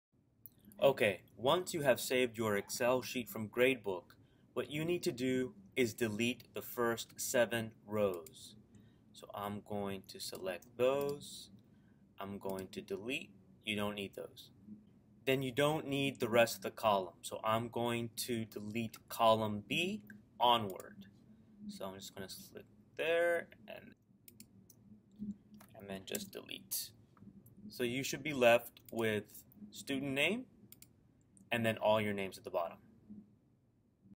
Mail Merge - Preparing your Excel file for mail merge
In this video, I am showing teachers who exported their class to Excel from Rediker's Gradebook how to prepare those Excel files for a Mail Merge.